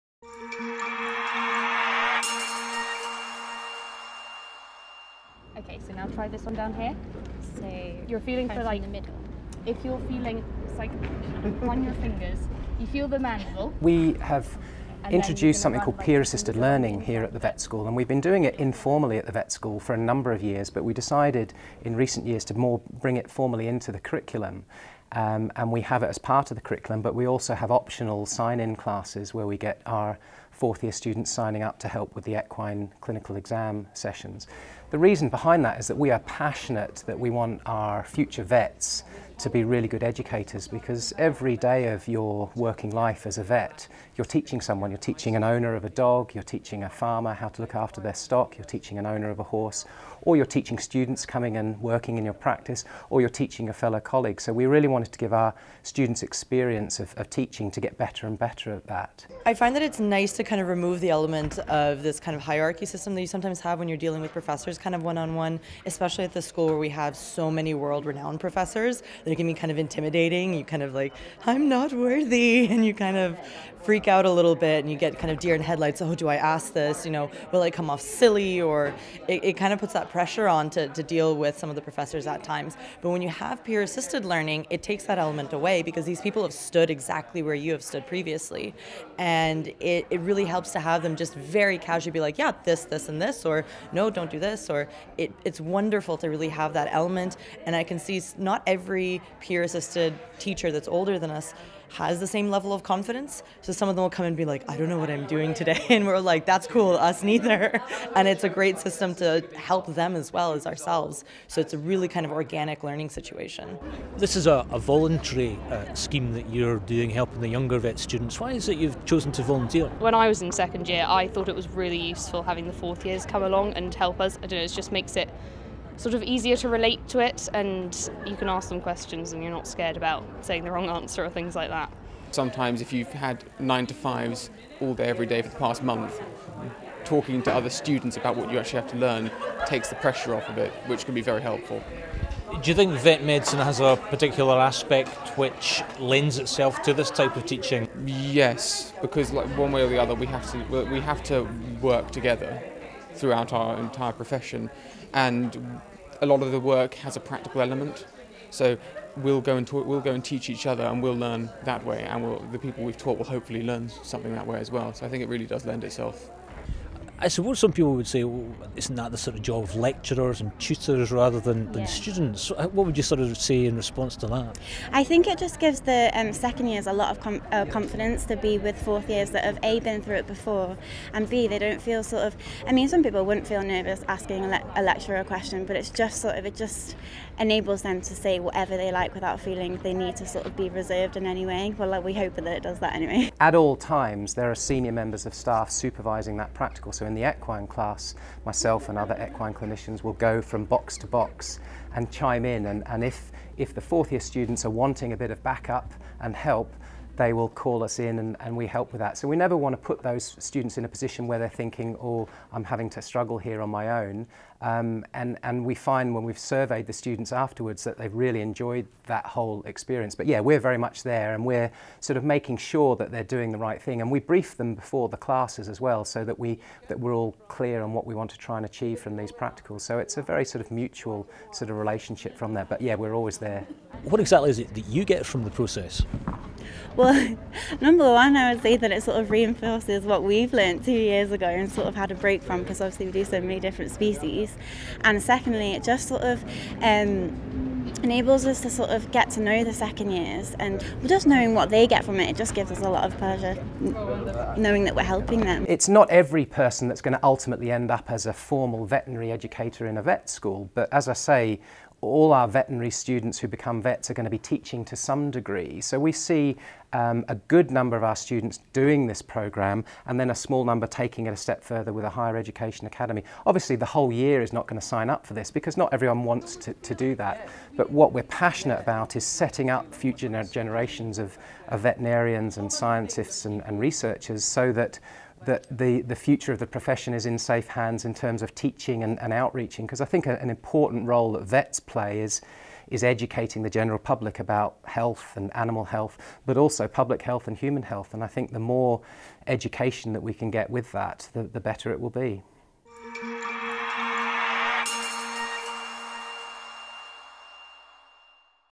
Teaching Matters Undergraduate Certificate in Veterinary Medical Education
Dr Neil Hudson explains the benefits of peer assisted learning in the Undergraduate Certificate of Veterinary Education.